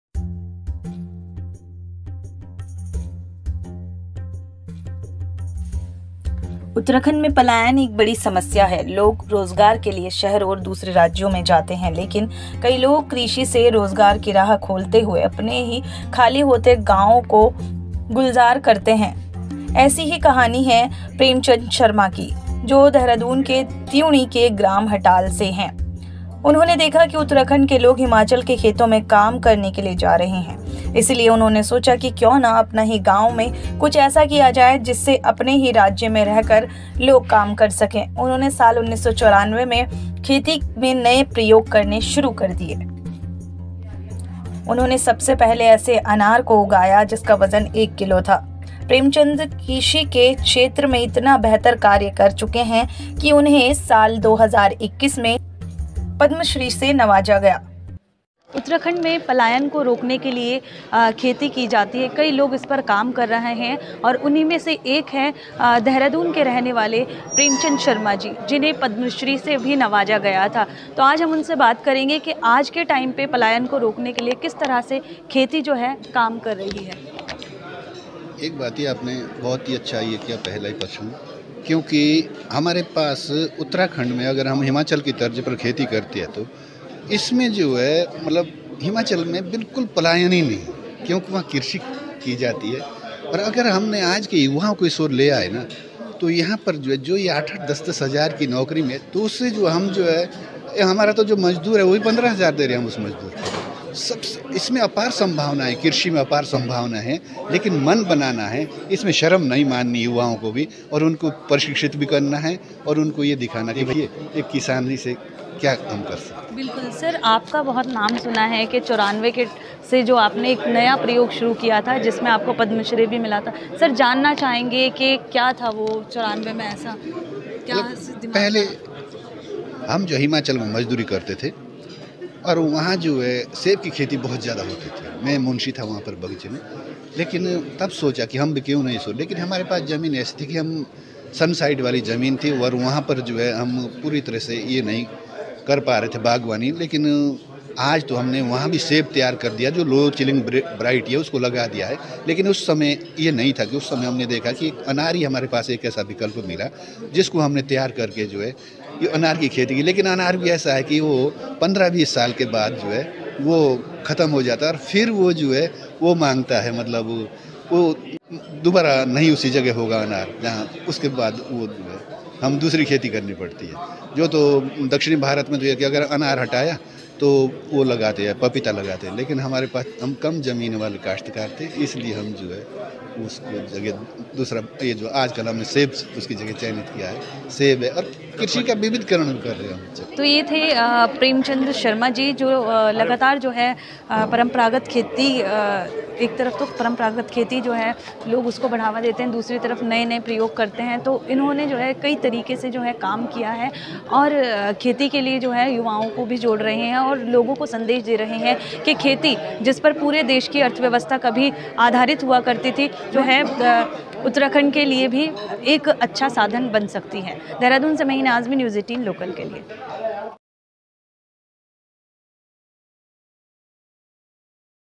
जज्‍बे को सलाम: पलायन रोकने के लिए खेती से लोगों को जोड़ रहे पद्मश्री प्रेमचंद्र, कृषि में करते हैं
उत्तराखंड के किसान पद्मश्री सम्मानित प्रेमचंद शर्मा देहरादून के त्यूणी के ग्राम हटाल से हैं. प्रेमचंद कृषि क्षेत्र में उत्कृष्ट कार्य करने के लिए पद्मश्री, किसान भूषण, कृषक सम्राट सम्मान, किसान सम्मान के अलावा कई पुरस्कारों से नवाजे जा चुके हैं.